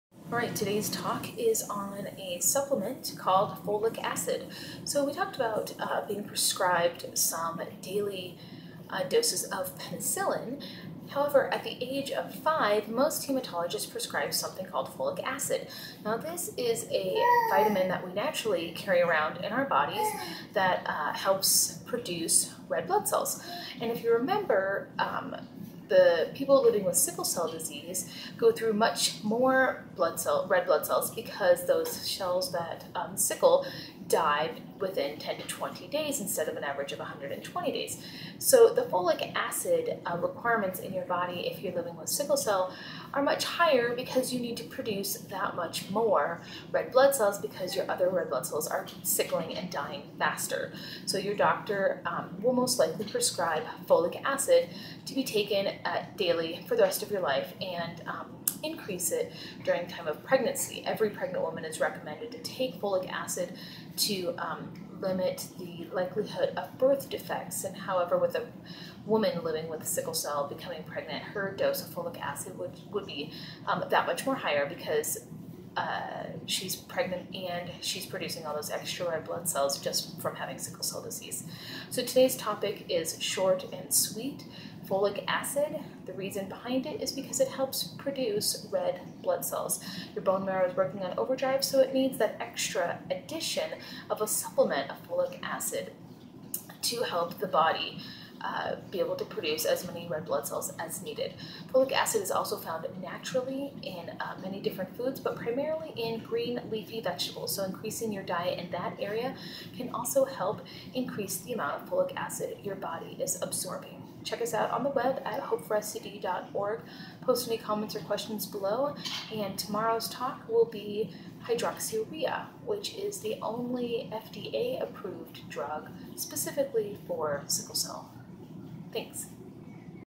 Folic Acid and Red Blood Cell Production
People living with Sickle Cell are often prescribed folic acid even though they don't technically have a deficiency. Why? Folic Acis helps red blood cell production!